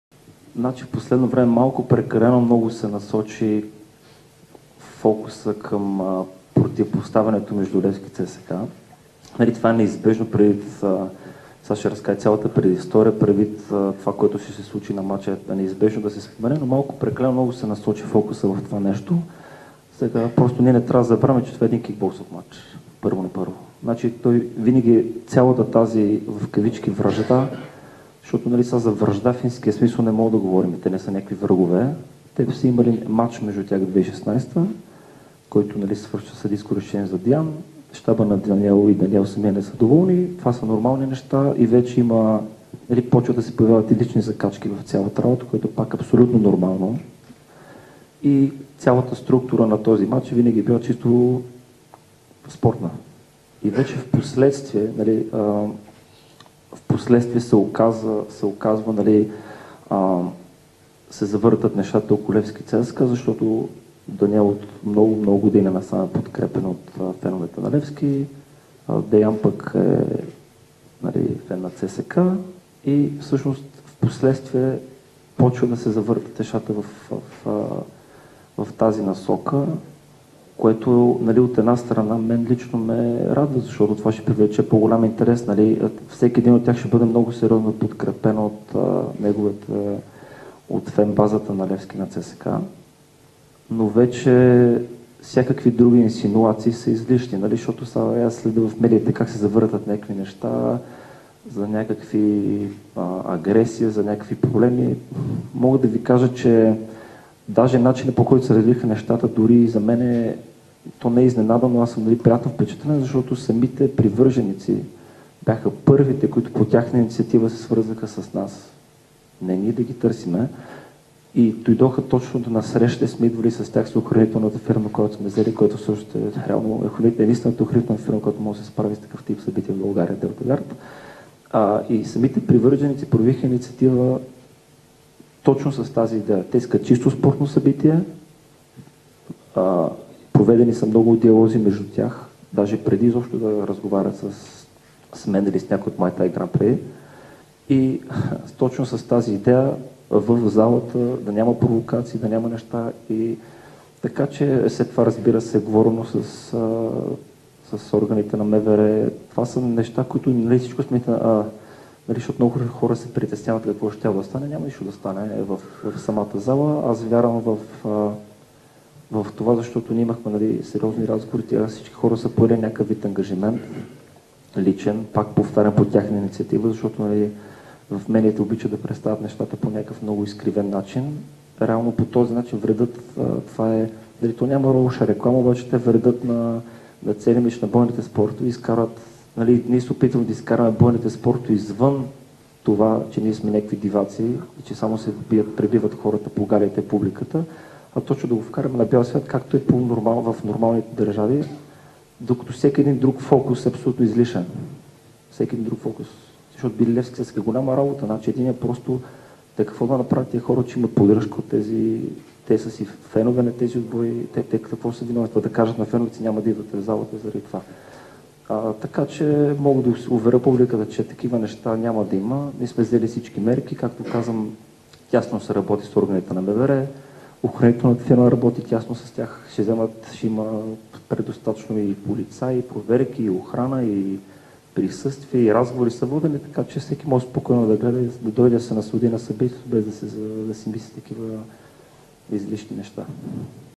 Александър Тосков: Радвам се, че феновете на Левски и ЦСКА ще подкрепят Илиев и Топалски
16 декември 2019 | 11:55